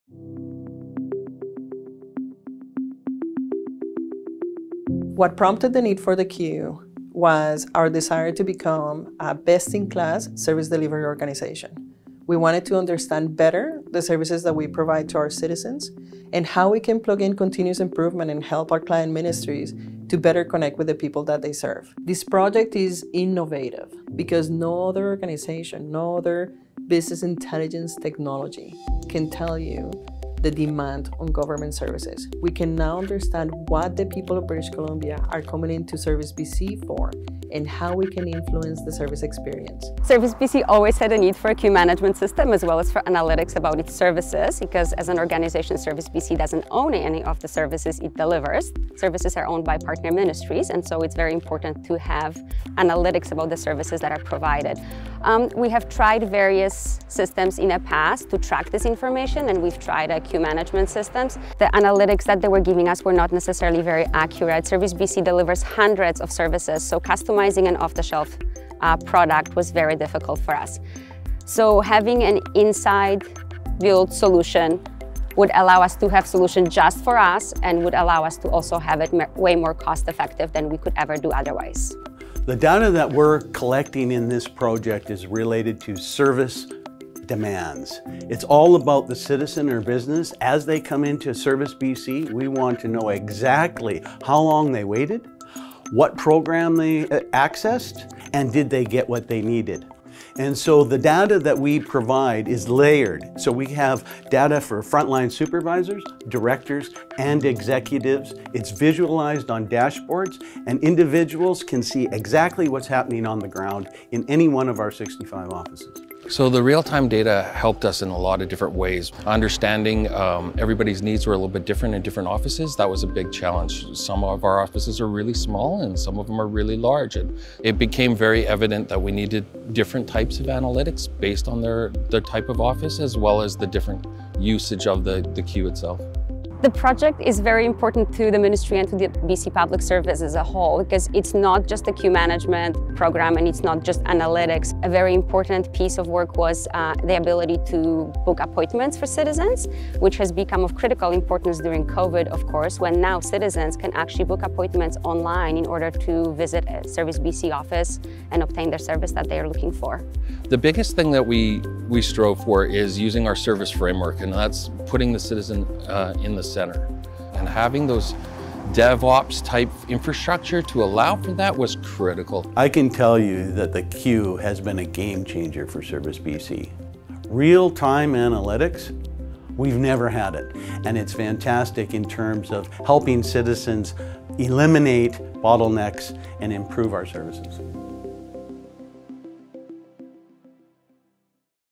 The Queue Customer Flow Management System
The Queue Customer Flow Management System (The Q) is a digital customer flow management system. Tailored for the needs of Service BC, Ministry of Citizens' Services designed The Q to provide real-time, evidence-based analytics. The Q is helping to maintain and improve the 96% satisfaction rating for in-person visits to Service BC locations.